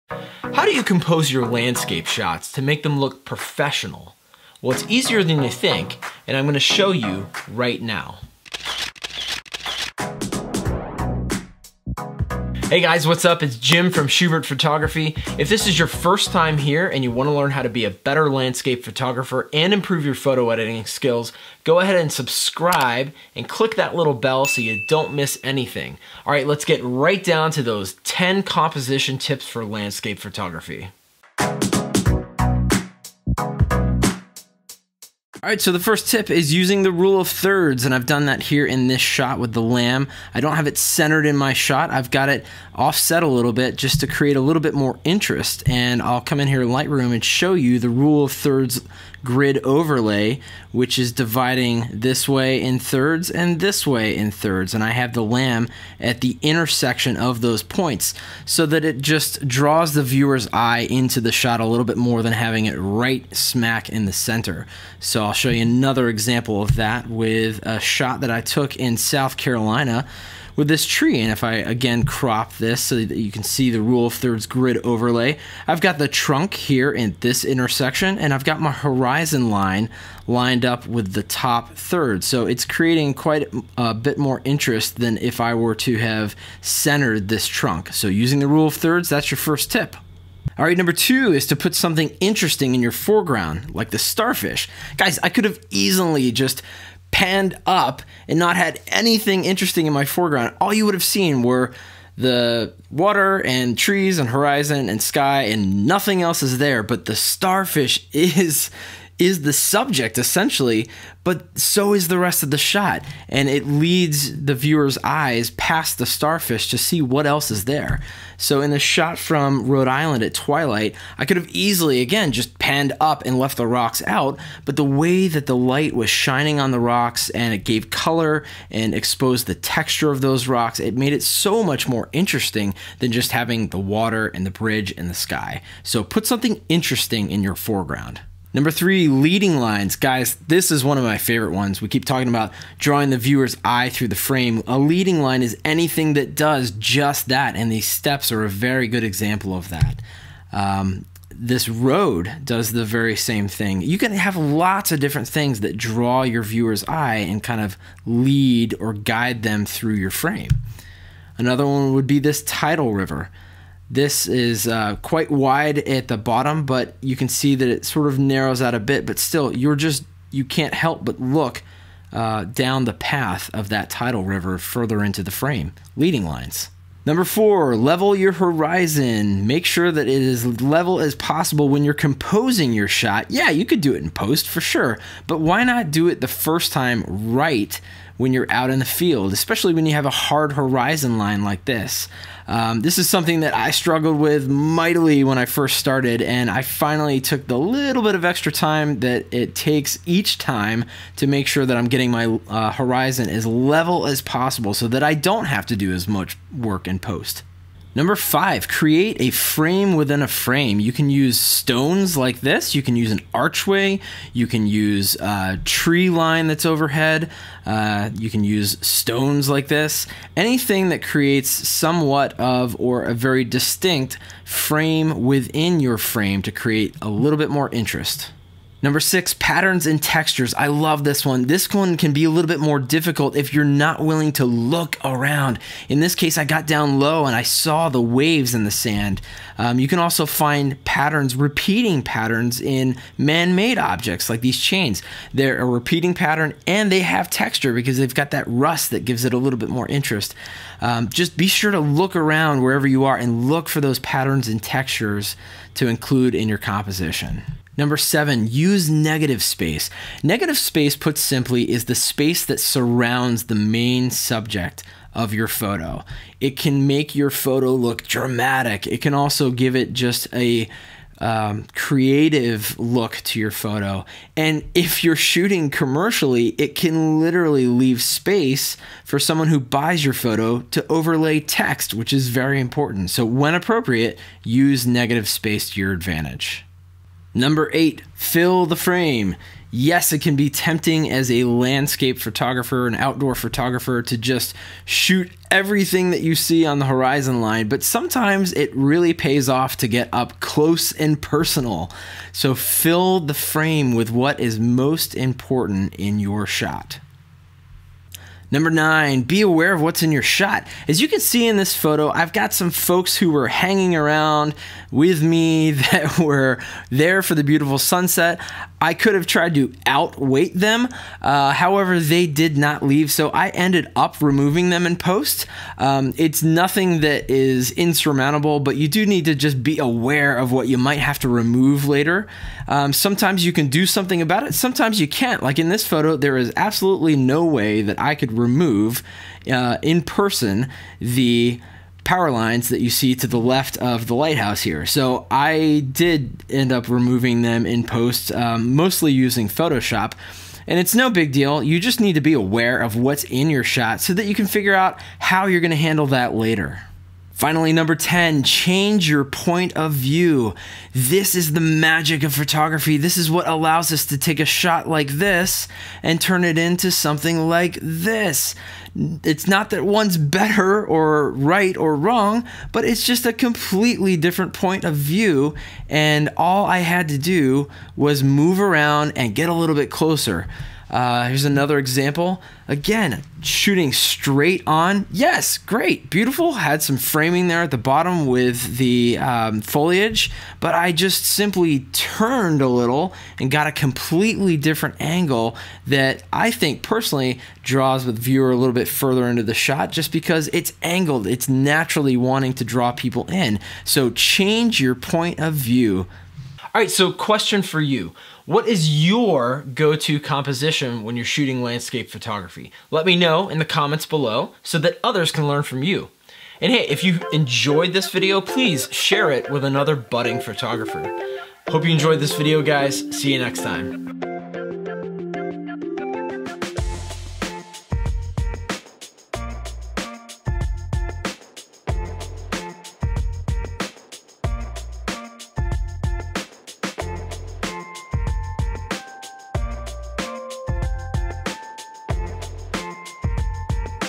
10 Essential Landscape Photography Composition Tips
These 10 landscape photography composition tips will help you create more impact in your shots.

EPISODE OVERVIEW:

One of the keys to improving your outdoor photography is to learn basic photography composition techniques. And that's exactly what we'll cover in this landscape photography tutorial.

10 Landscape Photography Composition Techniques

1. Rule of Thirds
2. Create Foreground Interest
3. Leading Lines
4. Level Your Horizon
5. Frame Within A Frame
6. Patterns & Textures
7. Negative Space
8. Fill The Frame
9. Be Aware Of What's In Your Shot
10. Change Your Point Of View